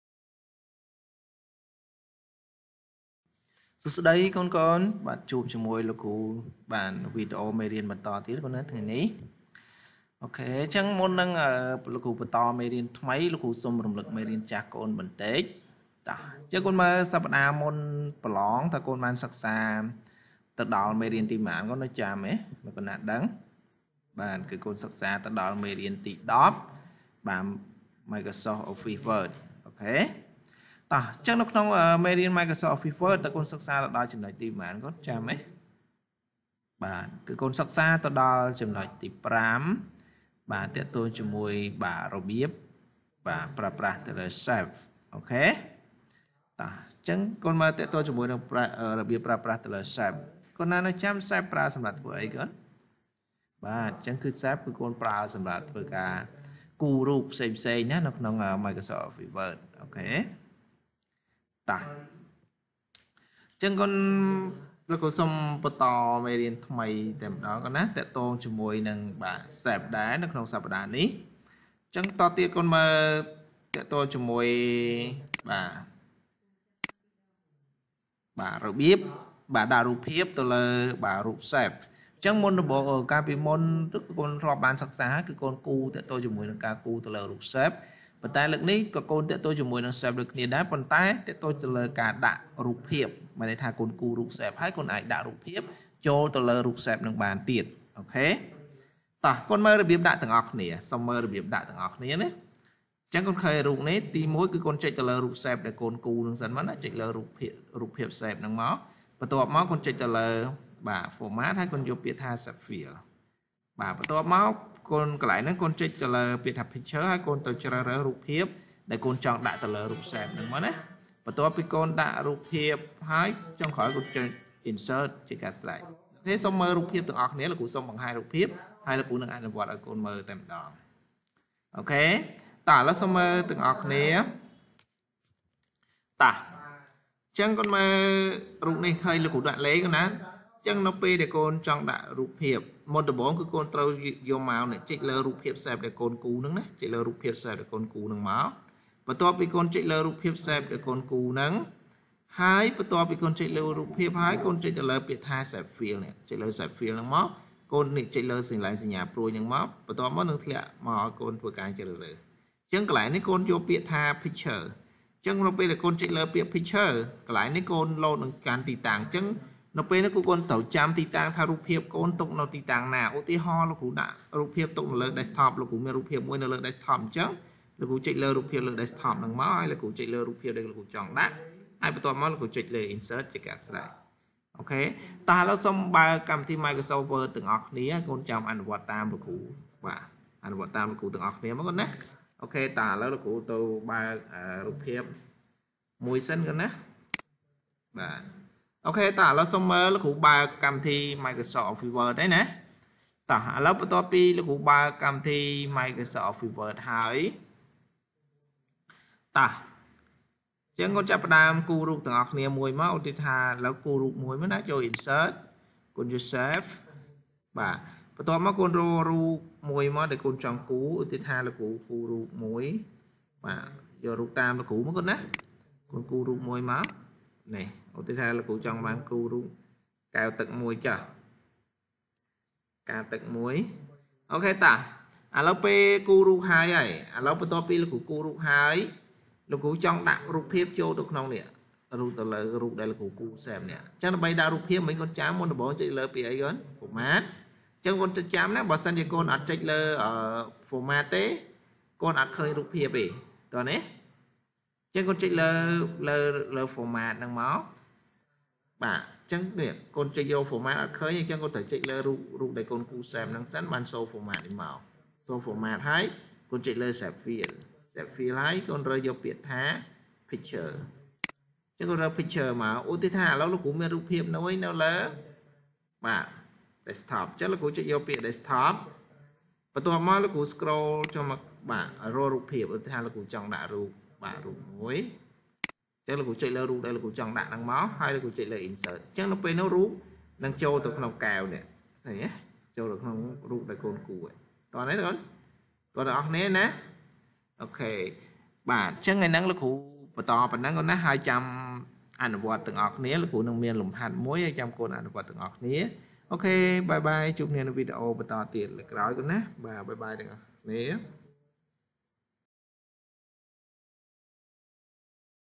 មេរៀនទី១០ Microsoft Office Word (ត) របៀបដាក់រូបភាពទៅលើរូប Shape
របៀបដាក់រូបភាពទៅលើរូប Shape